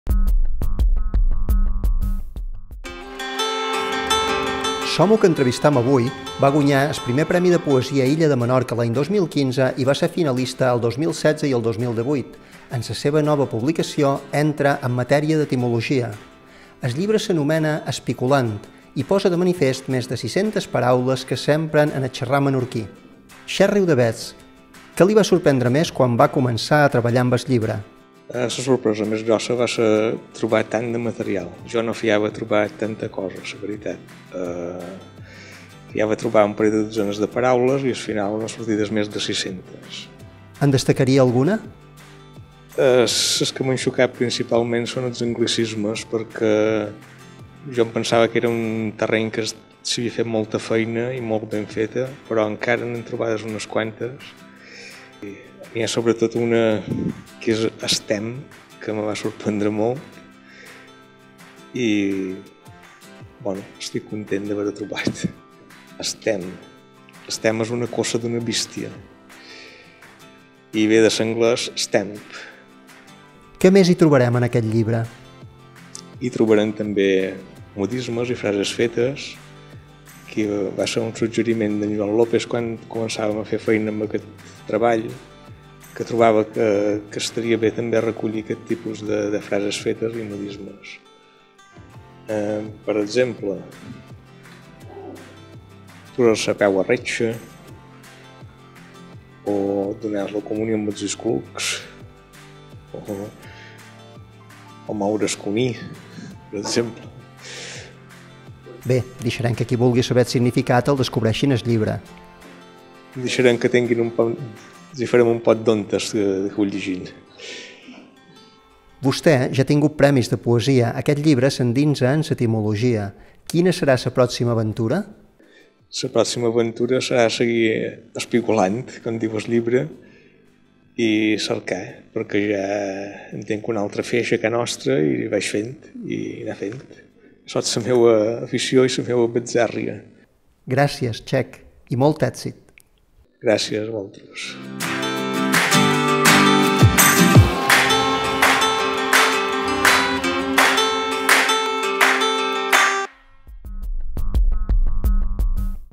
Xec Riudavets Autor del llibre ESPICOLANT
L’home que entrevistem avui, va guanyar el premi de poesia Illa de Menorca l’any 2015 i va ser finalista el 2016 i el 2018. En la seva nova publicació, entre en matèria d’etimologia. El Llibre sa nomena “Espicolant” i posa de manifest més de 600 paraules que s’empren en el xarrar Menorquí.